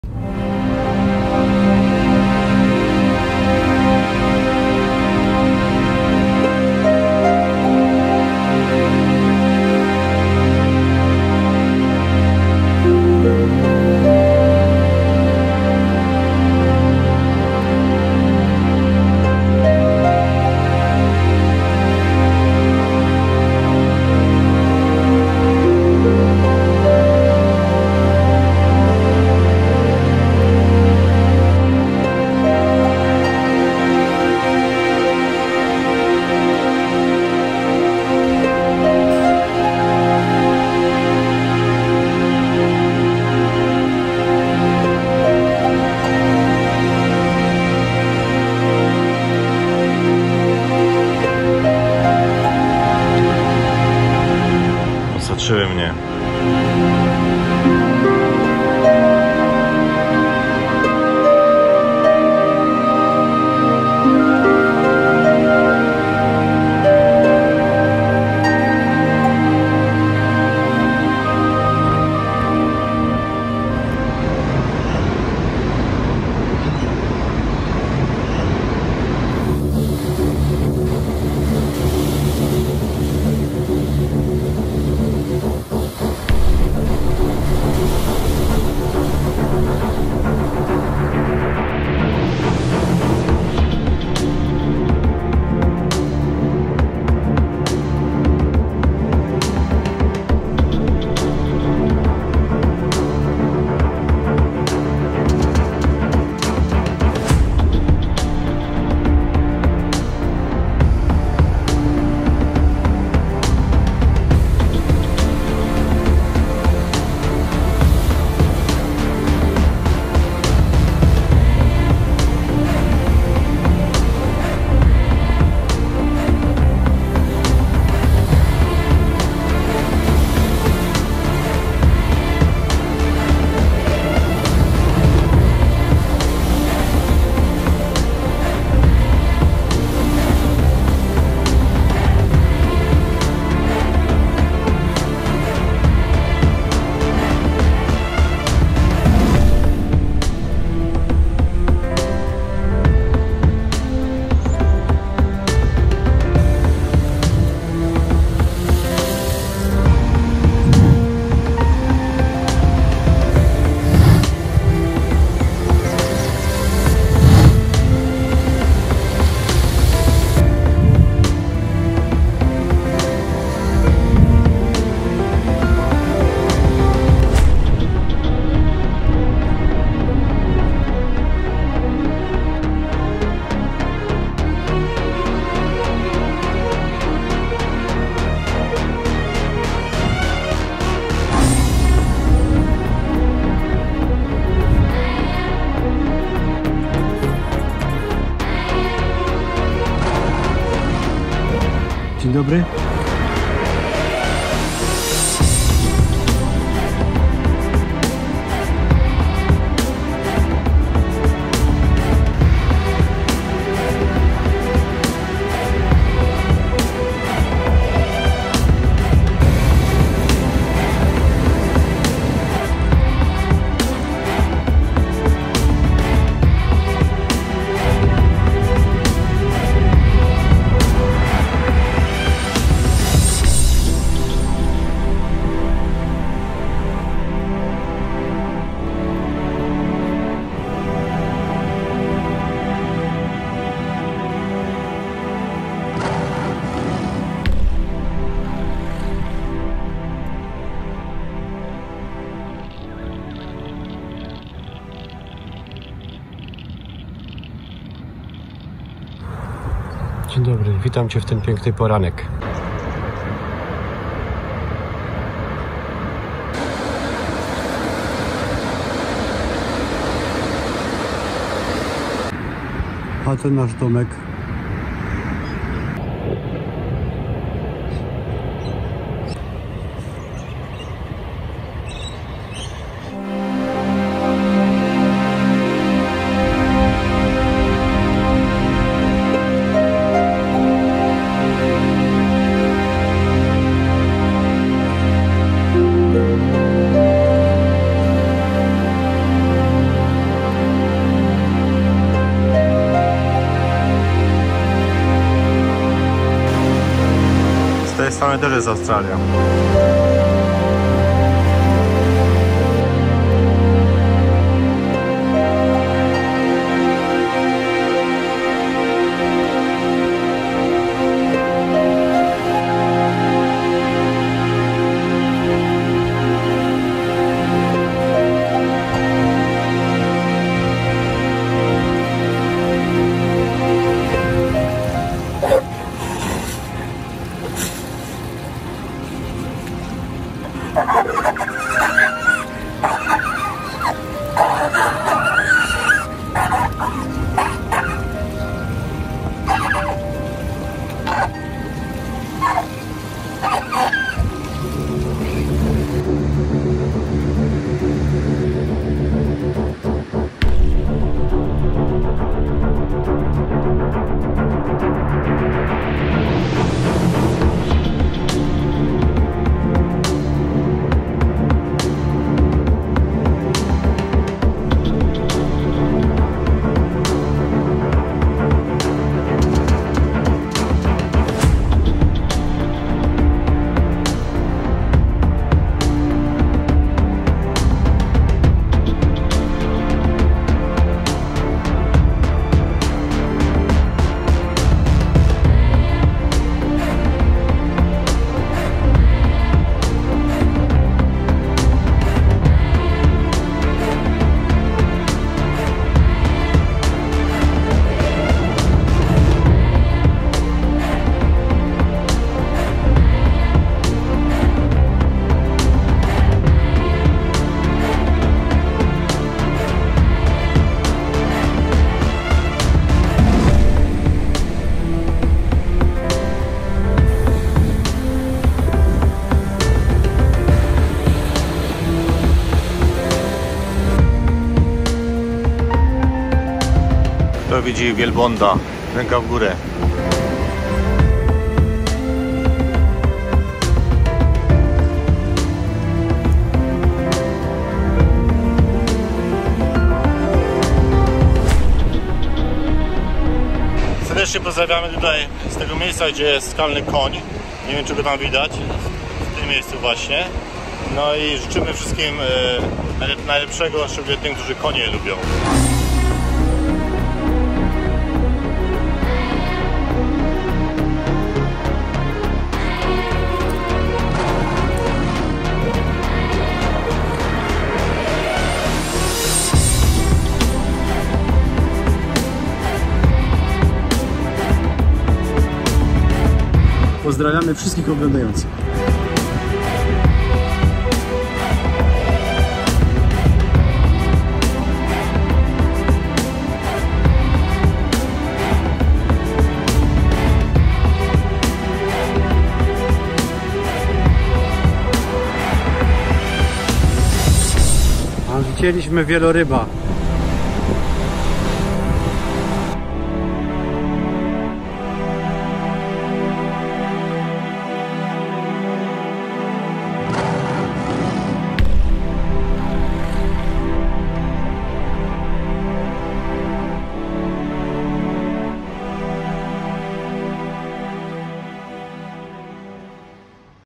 Sydney Merimbula
Pierwsza część podróży samochodem z Sydney do Melbourne. Fantastyczna przygoda! Zapraszamy!!!